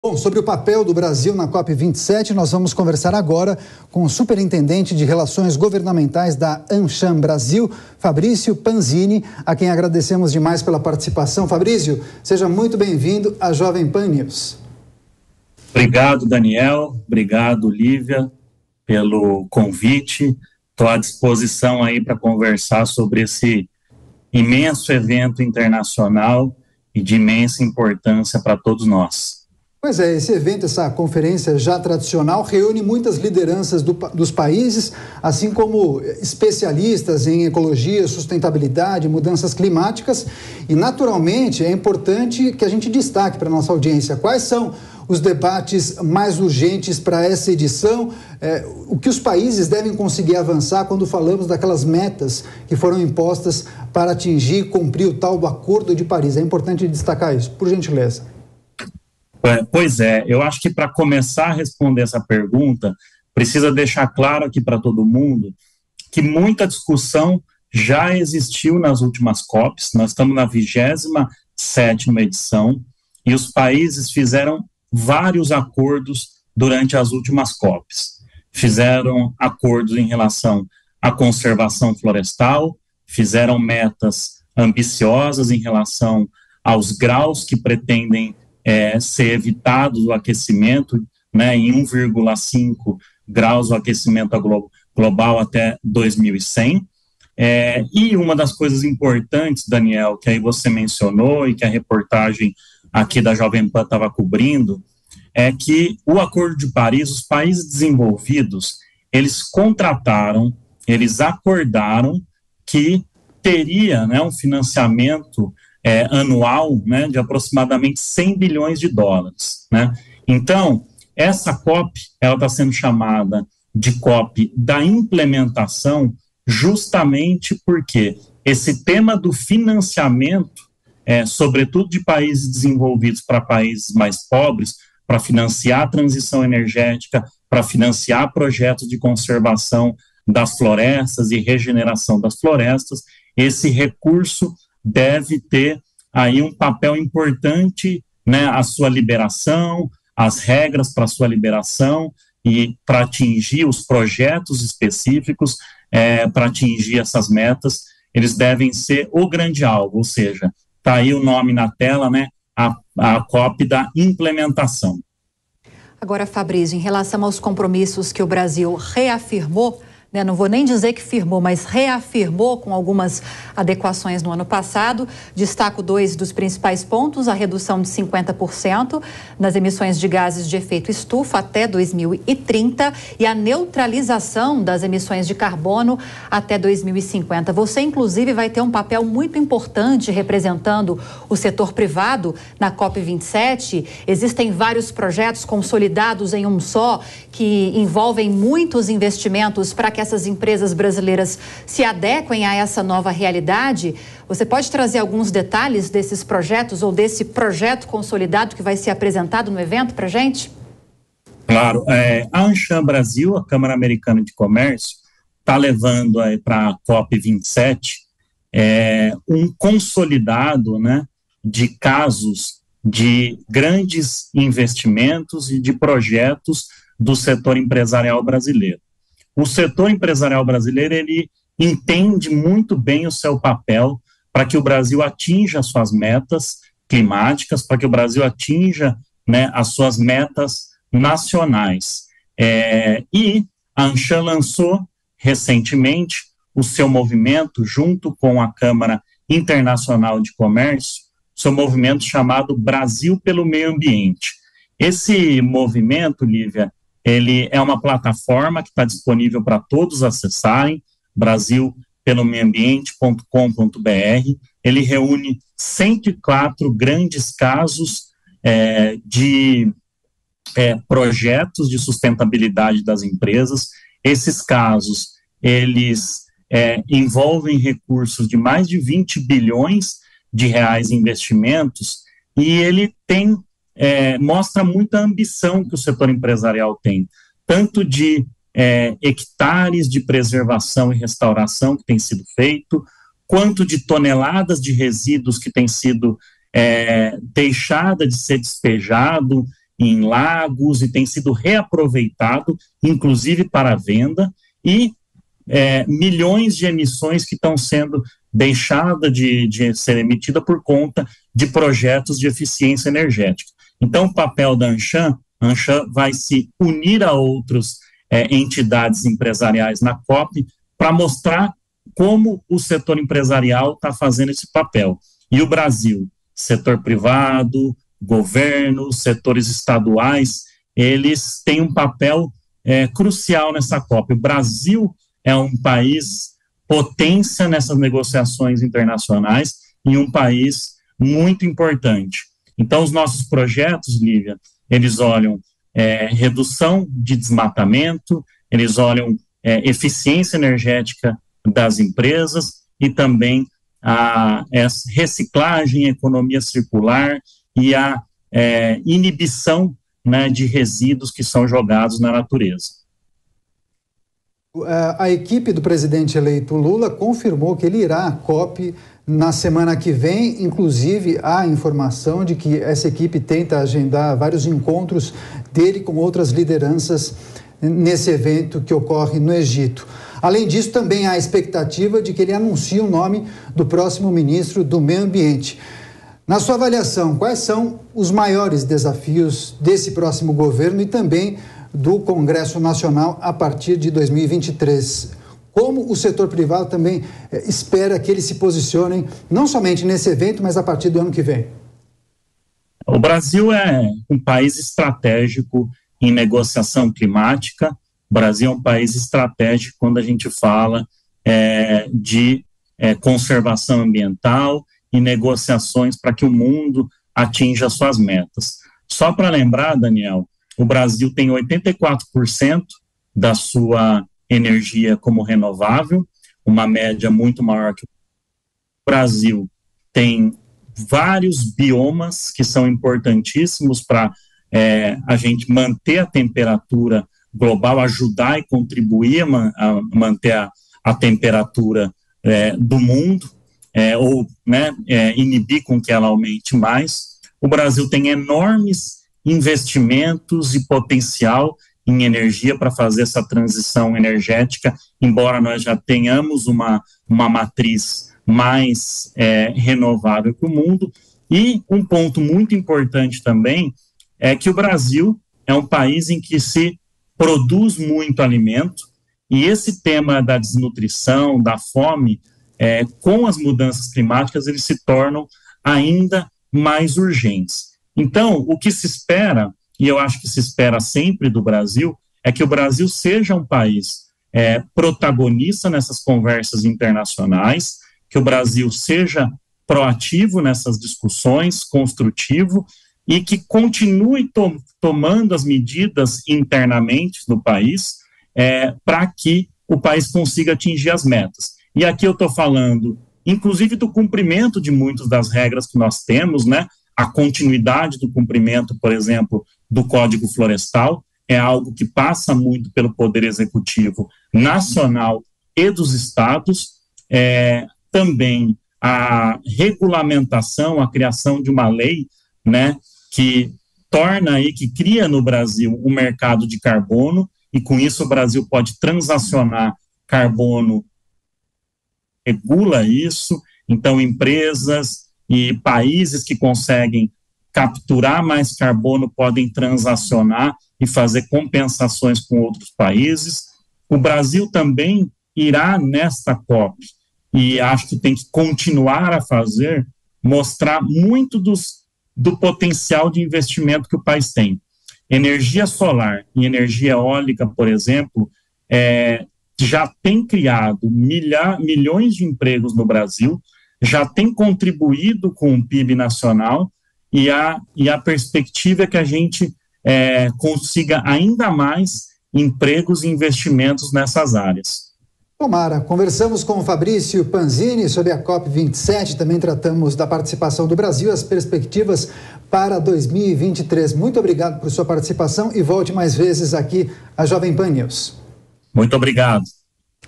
Entenda qual é o papel do Brasil nas negociações da COP 27
Fabrizio Panzini, superintendente de relações governamentais da Amcham Brasil, fala com exclusividade ao Jornal Jovem Pan. O projeto privado “Brasil Pelo Meio Ambiente” prevê investimentos de R$ 20 bilhões.